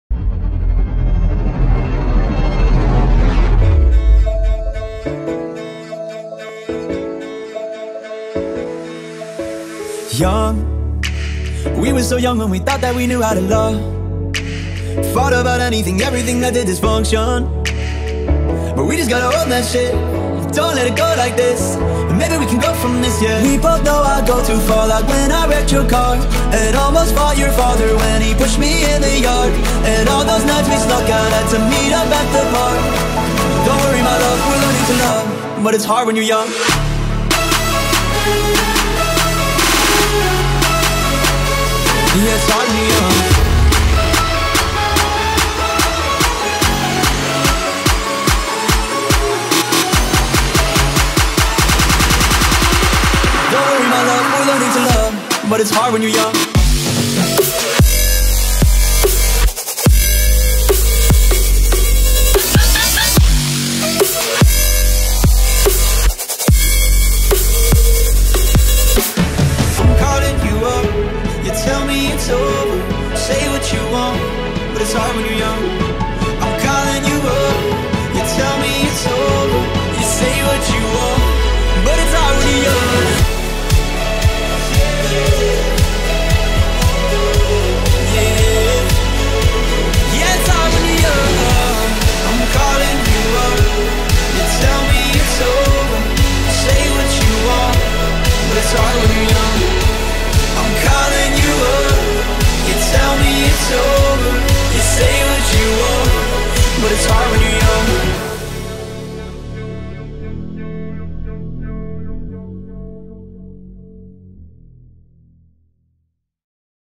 Young
Throwback to Lollapalooza Paris and Mainsquare Festival in Arras, it's all about love.